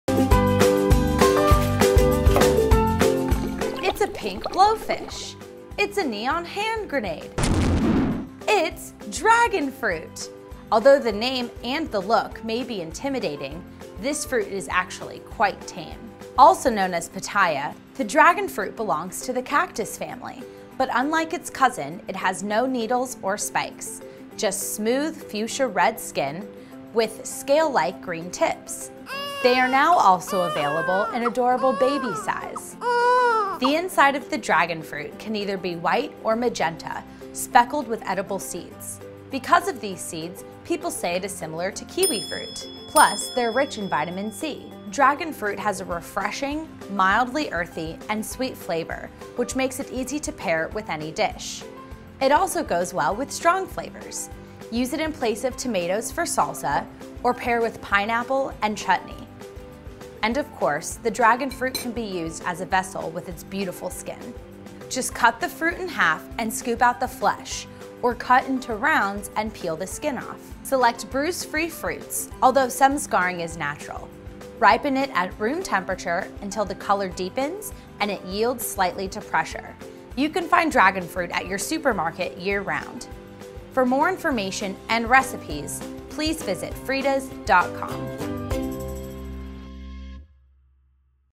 How to eat a Dragon Fruit
What is a Dragon Fruit? Also known as Pitaya, Dragon Fruit is a beautiful cactus family fruit with bright magenta or white flesh speckled with small, edible seeds. Excellent source of vitamin C--and a low FODMAP food--the Dragon Fruit has a mild and slightly earthy flavor. The texture is often compared to kiwifruit. Although it is the fruit of a cactus, it does not have any spines or needles on the outer skin (unlike its cousin, the Cactus Pear).

Dragon Fruit is easy to peel—cut in half lengthwise and scoop out the flesh, or cut into rounds and peel. Enjoy alone, or add to salsa, fruit salads, smoothies, or cocktails. The mild flavor also pairs well with stronger flavors like pineapples and strawberries. The beautiful, unique skin can be used as a serving vessel.

Select bruise-free Dragon Fruits, although some scarring is common, and store refrigerated. These gorgeous tropical fruit is available year round.

Love Dragon Fruit? Share your photo and story with us!